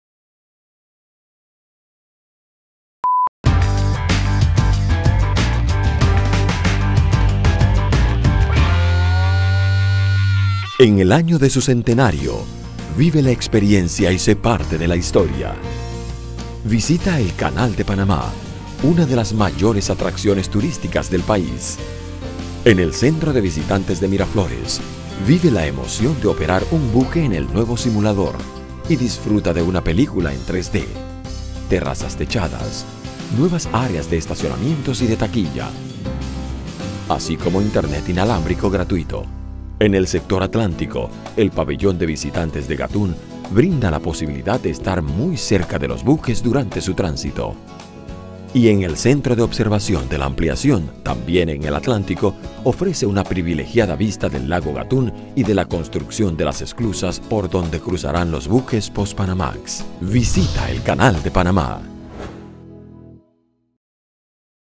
¡Conoce el Canal de Panamá rumbo a su Centenario!
¿Te imaginas el tamaño de los buques que surcan los océanos?

Desde el Centro de Visitantes de Miraflores y Centro de Visitantes de las Esclusas de Gatún vivirás la experiencia que después de casi 100 años aún fascina al mundo: el Canal de Panamá.